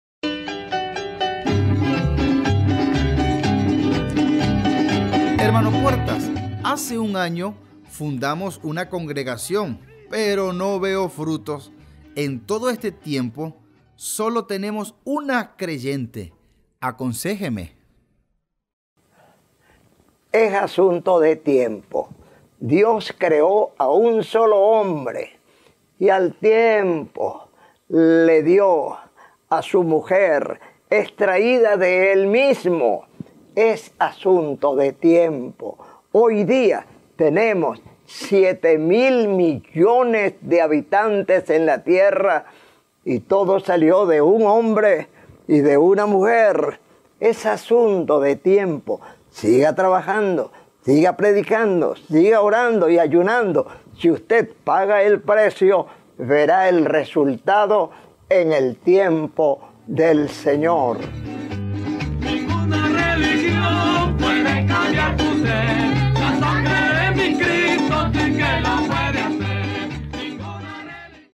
HNO PUERTAS RESPONDE #084 | NO VEO FRUTOS EN MI CONGREGACIÓN ¿QUE HAGO? | MICRO HNO PUERTAS | LUZ TV
pastor Jaime Puertas:
 ¿HACE UN AÑO FUNDAMOS UNA CONGREGACIÓN, PERO NO VEO FRUTOS EN TODO ESTE TIEMPO, SOLO TENEMOS UNA CREYENTE ACONSÉJAME?